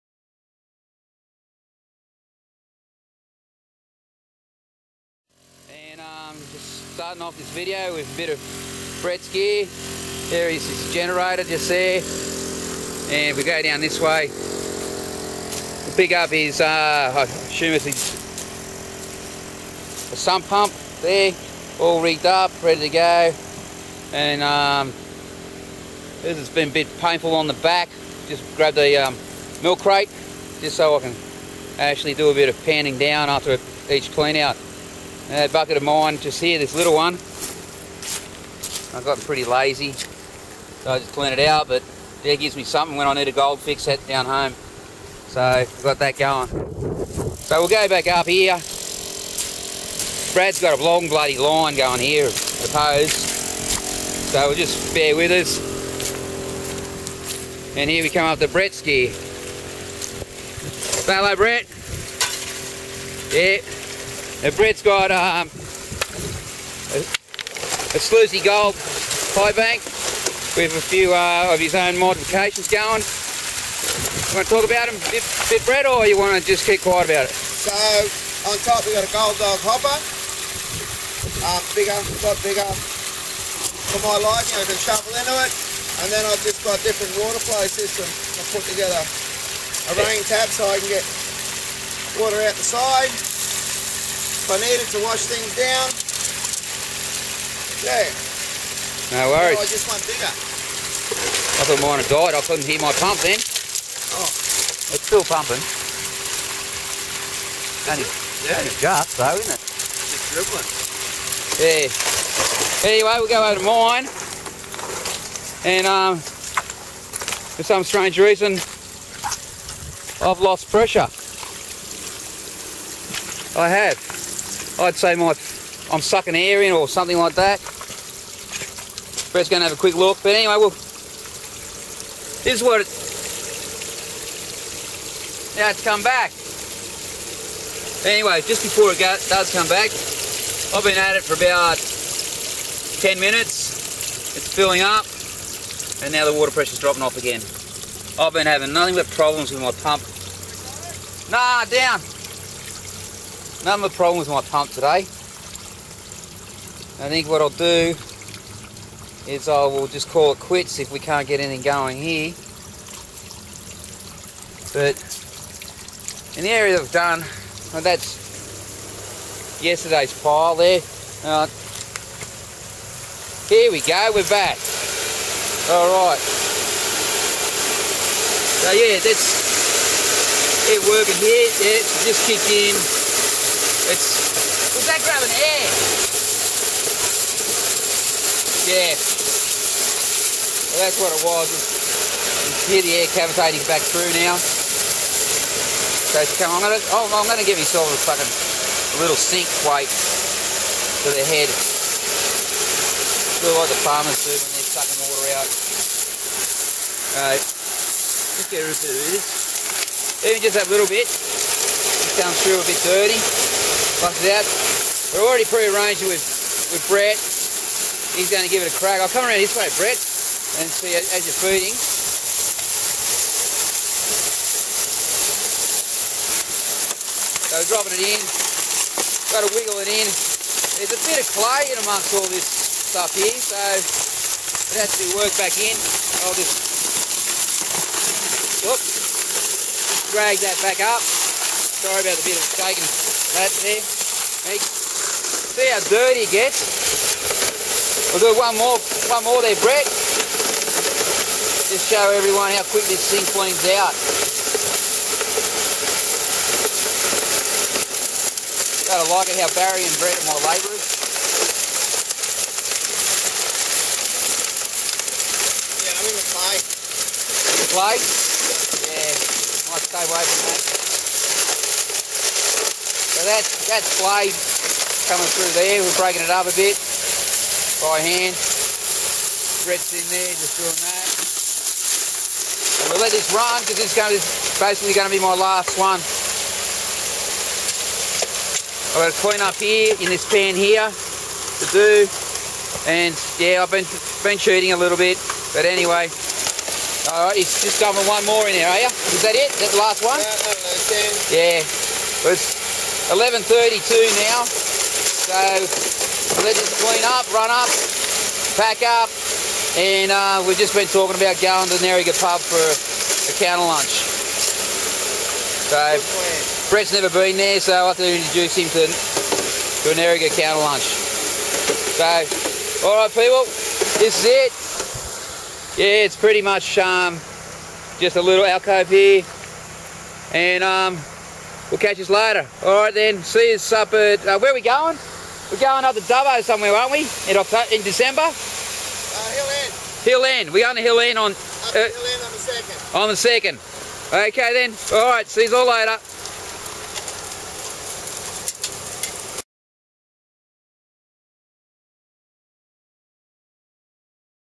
Another weekend at Oallen
Had a free weekend and put the call out there if anyone was keen for a bit of highbanking. Brett Edwards answered the call and came all the way out from Orange, a 4 hour trip to get there. I am sure that he had a good experience of Oallen gold, albeit small in size.